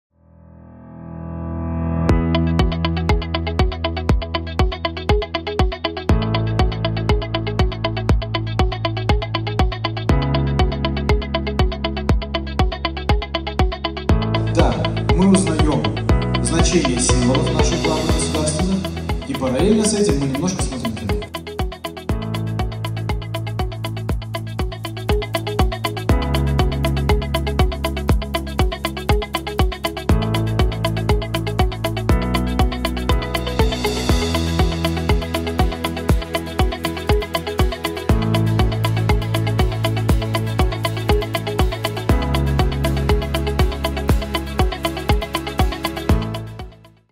В Гомельском областном управлении состоялась премьера фильма "Культурный код"
Сегодня в актовом зале областного управления МЧС для спасателей области состоялась премьера уникального кинопроекта «Культурный код». Более того, особенность мероприятия подчеркнуло присутствие режиссёра фильма - Кирилла Халецкого и главной героини - Дарьи Лазарчик.